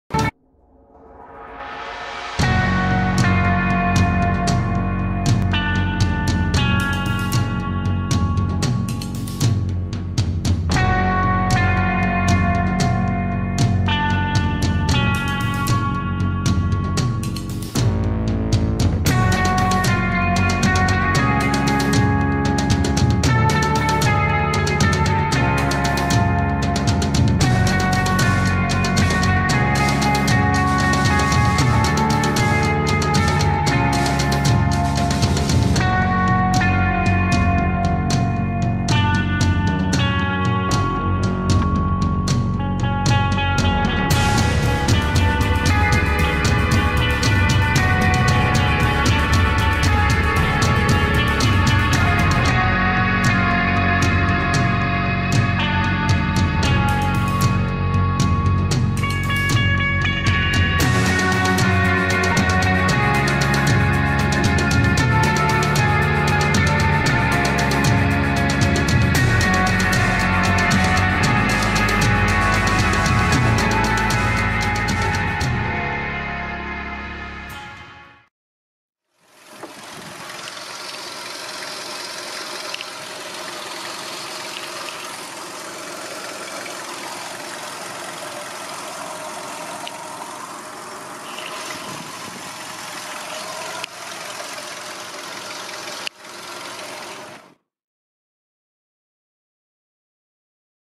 Autokomis Soloch Oferta sprzedaży: VW Golf V 1.6 102KM 2010 -Prezentacja. Goczałkowice-Zdrój
Volkswagen Golf V 1.6 102KM 3Drzwi -Klima. Rok: 2008. Przebieg: 119 tys km. Cena jak w ogłoszeniu:

Nasz profil na FB Dźwięk silnika - praca silnika

Autohandel
Car trade
Commerce automobile
Торгівля автомобілями
Торговля автомобилями
تجارة السيارات